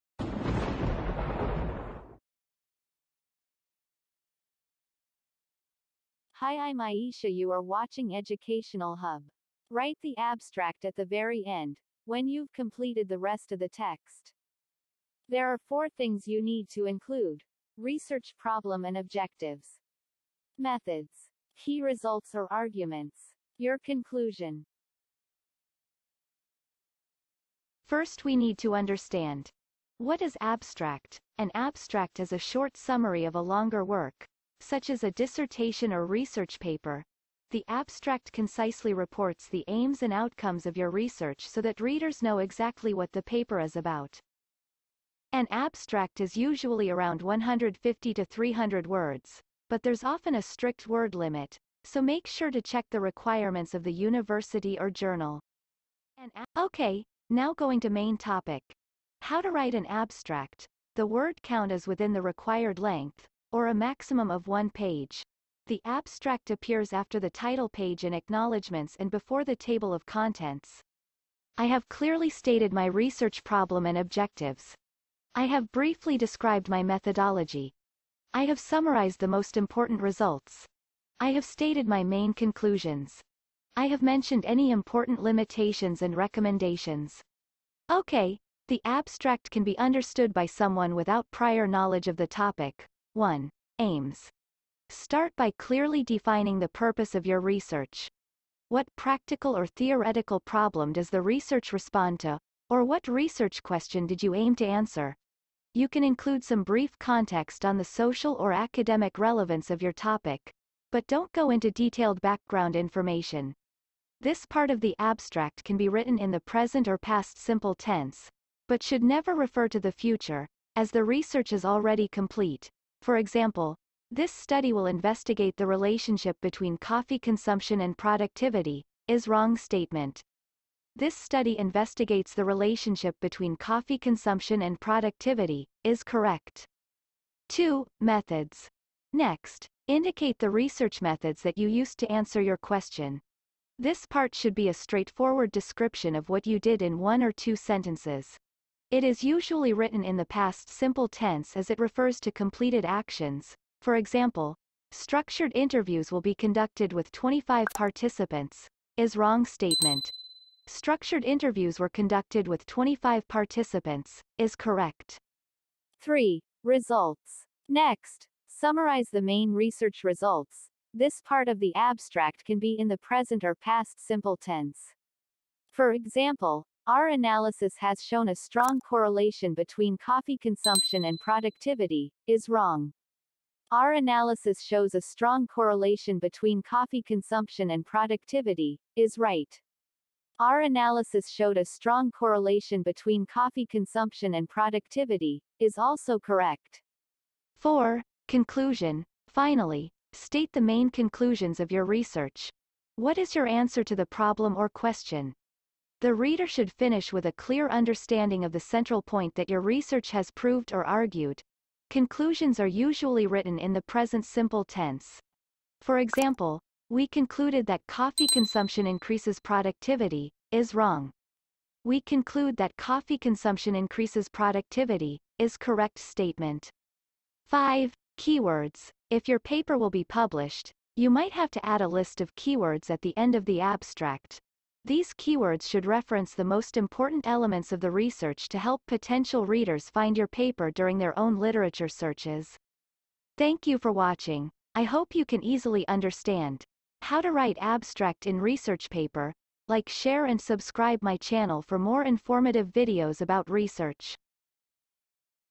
how to write an abstract for a research paper l what is an abstract l step by step guide
An abstract is a brief summary of a research article, thesis, review, conference proceeding, or any in-depth analysis of a particular subject and is often used to help the reader quickly ascertain the paper's purpose.
Links,
Blogspot.